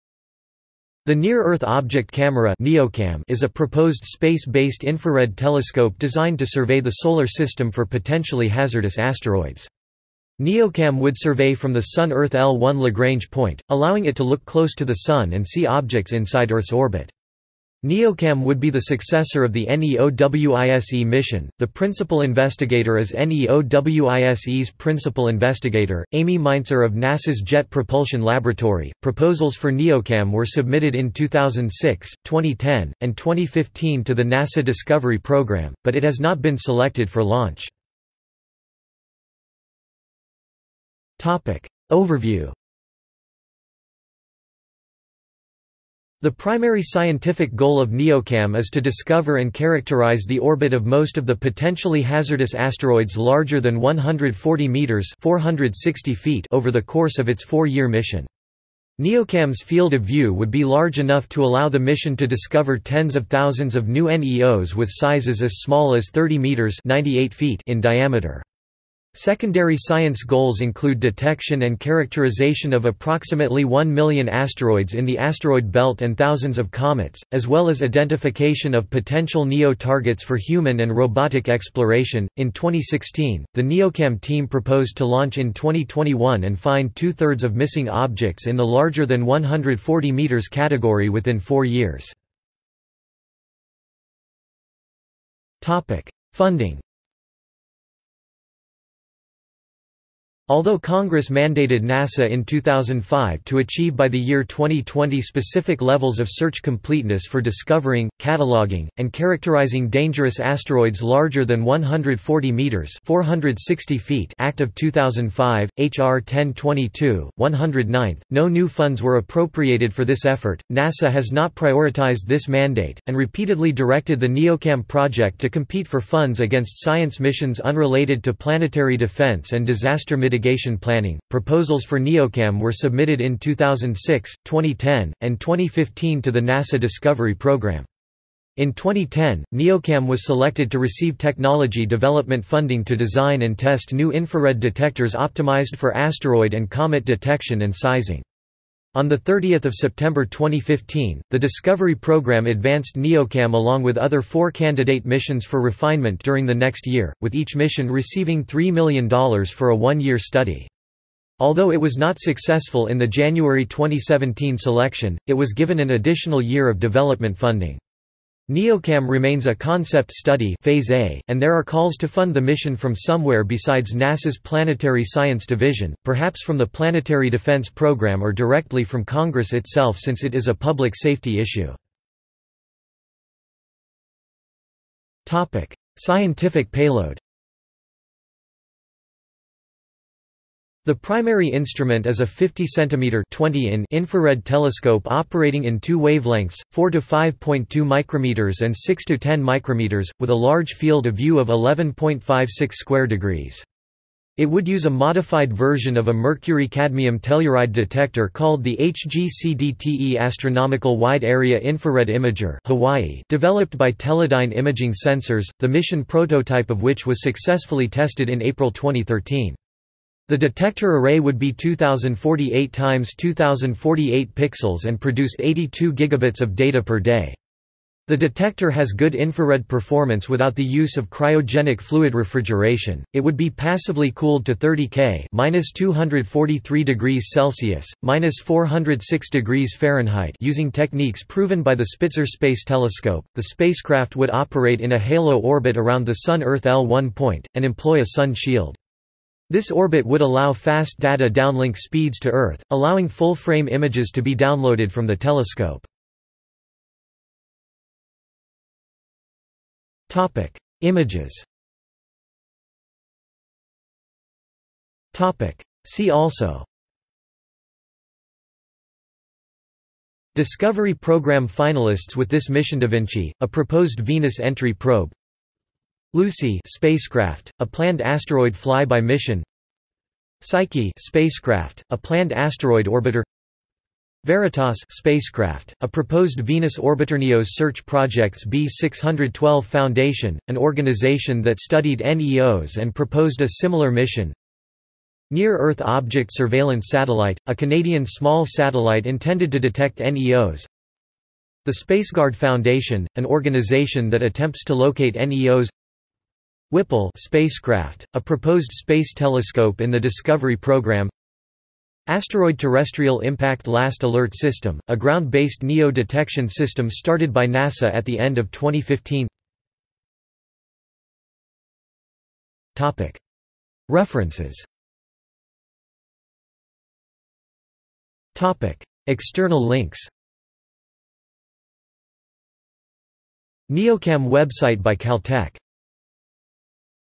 Near-Earth Object Camera | Wikipedia audio article
This is an audio version of the Wikipedia Article:
Near-Earth Object Camera

- Socrates

SUMMARY
The Near-Earth Object Camera (NEOCam) is a proposed space-based infrared telescope designed to survey the Solar System for potentially hazardous asteroids.  NEOCam would survey from the Sun–Earth L1 Lagrange point, allowing it to look close to the Sun and see objects inside Earth's orbit. NEOCam would be the successor of the NEOWISE mission; the principal investigator is NEOWISE's principal investigator, Amy Mainzer of NASA's Jet Propulsion Laboratory.Proposals for NEOCam were submitted in 2006, 2010, and 2015 to the NASA Discovery Program, but it has not been selected for launch.